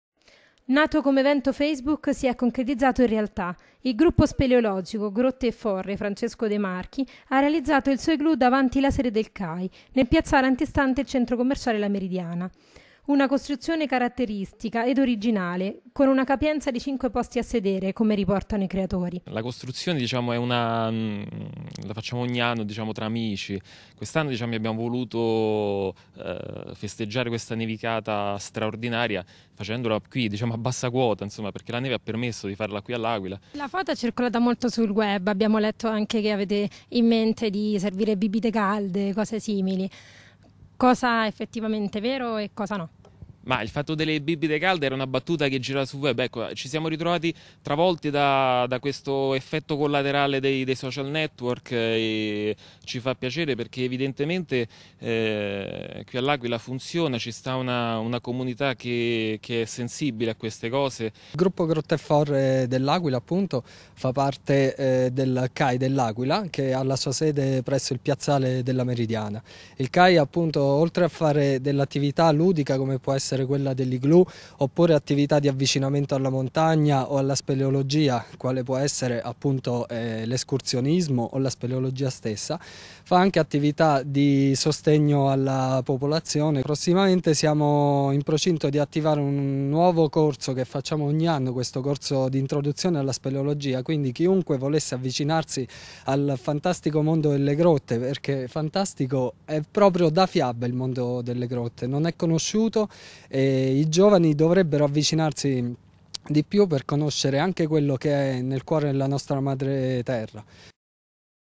L'IGLOO DEL GRUPPO SPELEOLOGICO DI L'AQUILA
TVUNO L'AQUILA - Il gruppo speleologico Grotte e Forre "F. De Marchi" L'Aquila, ha creato davanti la sede del CAI un igloo. Un idea originale che ha spopolato sul web.